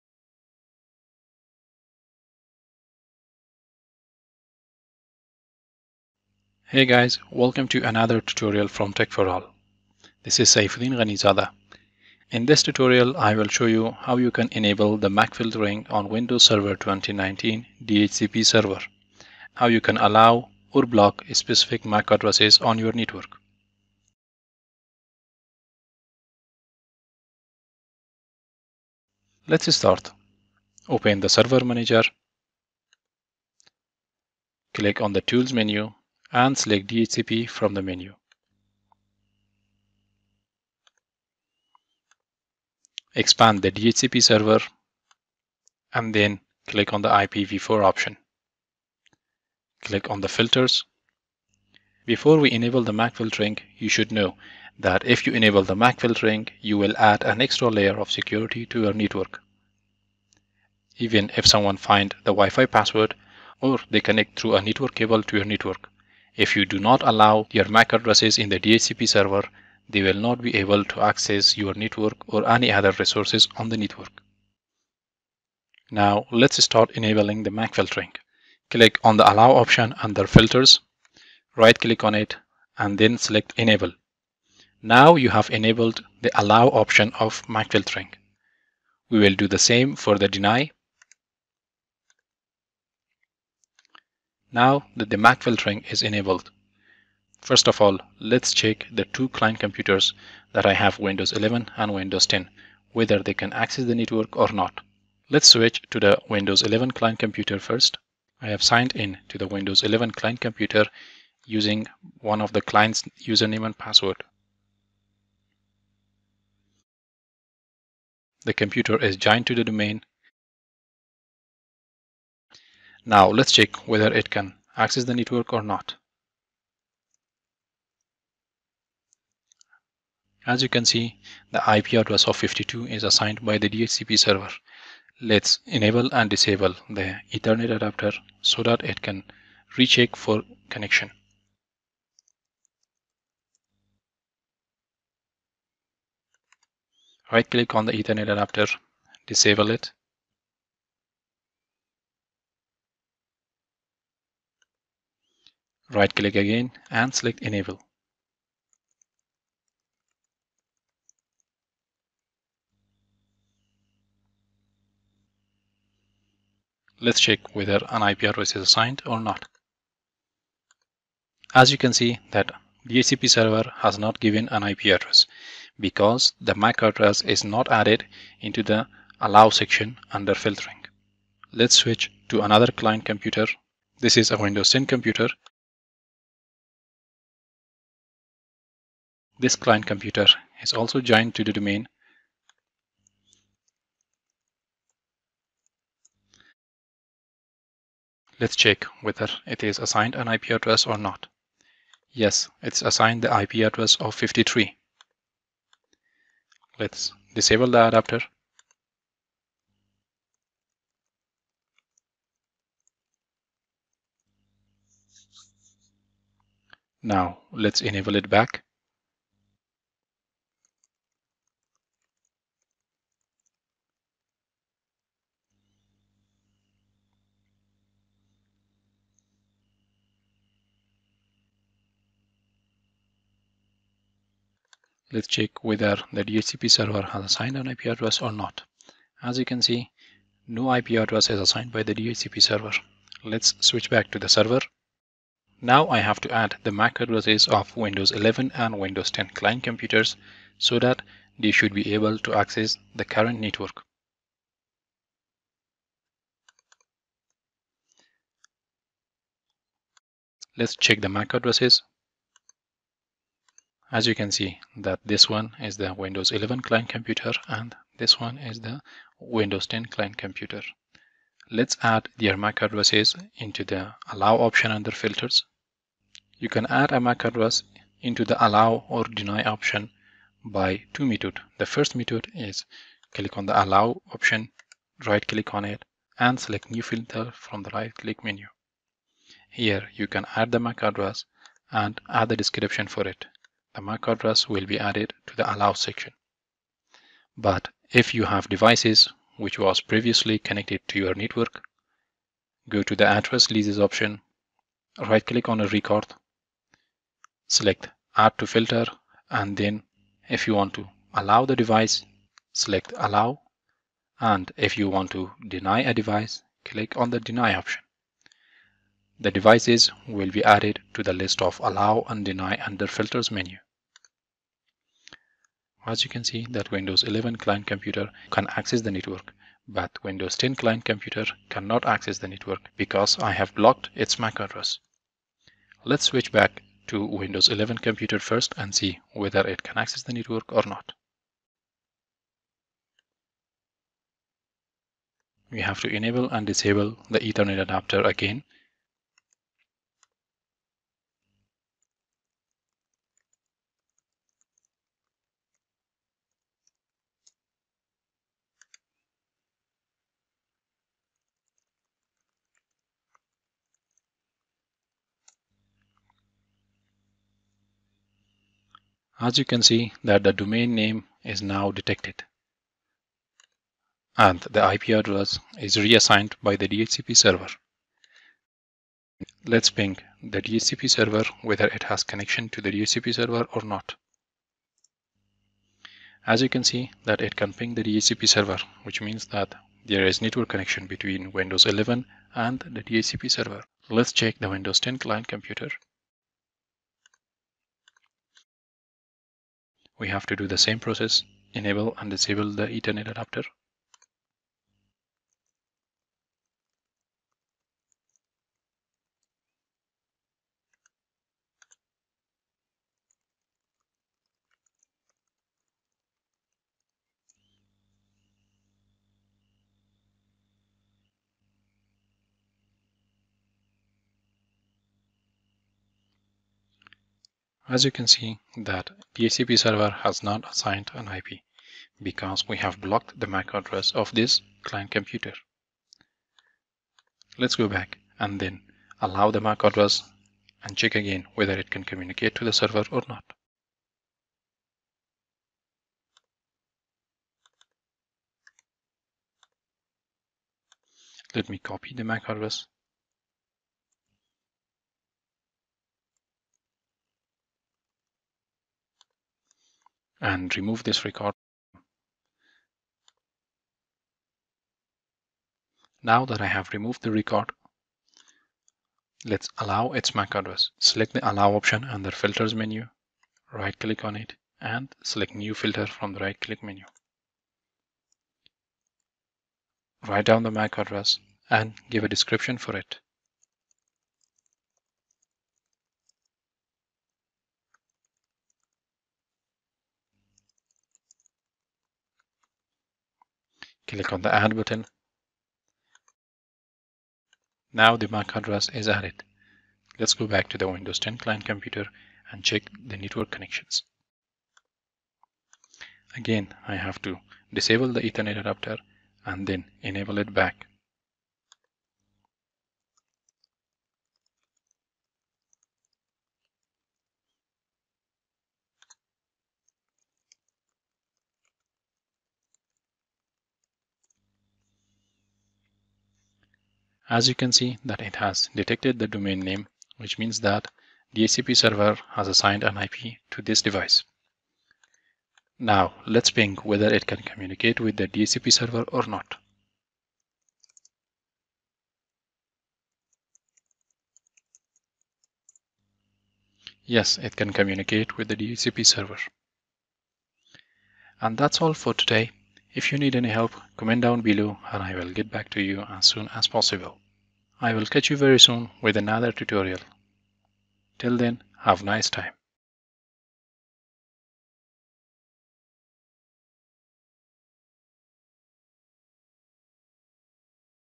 15- Windows Server 2019 Enabling Mac Filtering in DHCP Server
Hey Guys,
Welcome to another tutorial on Windows Server 2019. In this tutorial, I will show you guys how you can enable the Mac filtering, Reserve an IP address, Allow a specific mac address and block a specific mac address.

𝙏𝙞𝙢𝙚𝙨𝙩𝙖𝙢𝙥𝙨/𝙏𝙞𝙢𝙚𝙘𝙤𝙙𝙚𝙨
0:00 Intro
0:24 Timestamps/Timecodes
0:29 Access DHCP manager
1:15 Enabling mac filtering
1:37 Checking Windows 11 Computer for network connection
3:03 Checking Windows 10 Computer for network connection
4:05 Adding mac address to allow & deny list of mac filtering
5:41 Checking Windows 11 client computer after mac filtering is enabled
6:36 Checking Windows 10 client computer after mac filtering is enabled
7:24 Deleting a record from deny list of mac filtering
7:48 Adding a mac address in the allowed list of mac filtering